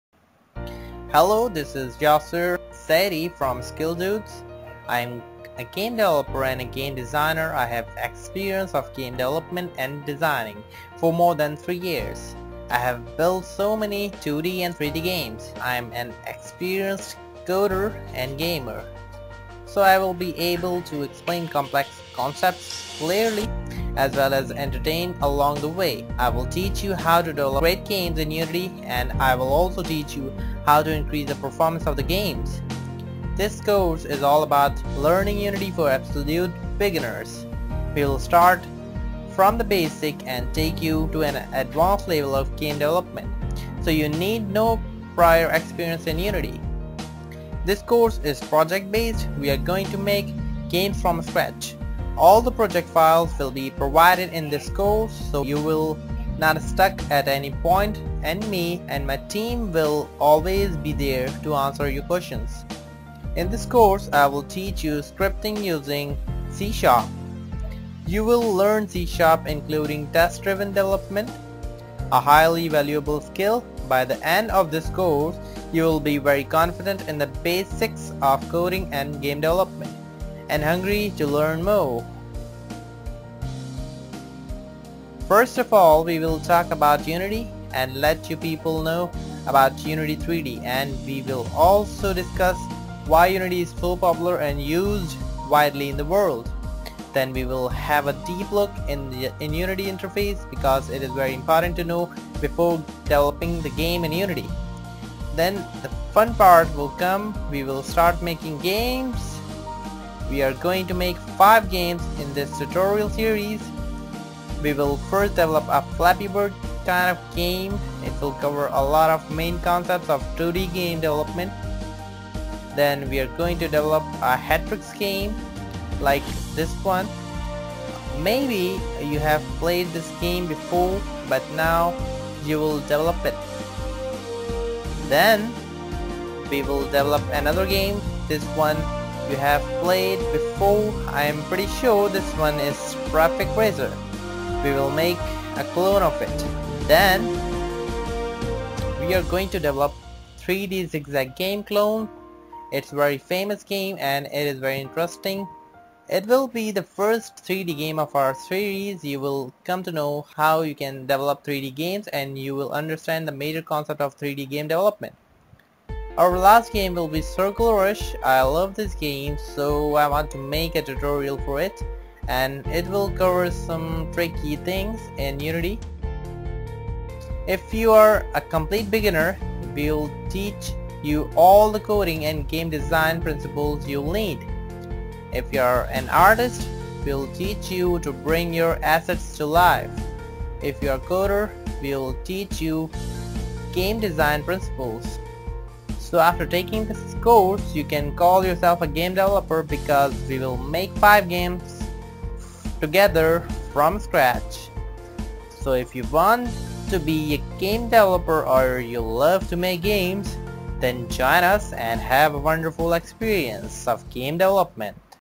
Start learning unity3d by making 5 games from scratch
This course is all about learning unity for absolute beginners. We will start from the very basic and take you to an advance level of game development so you need no prior experience in unity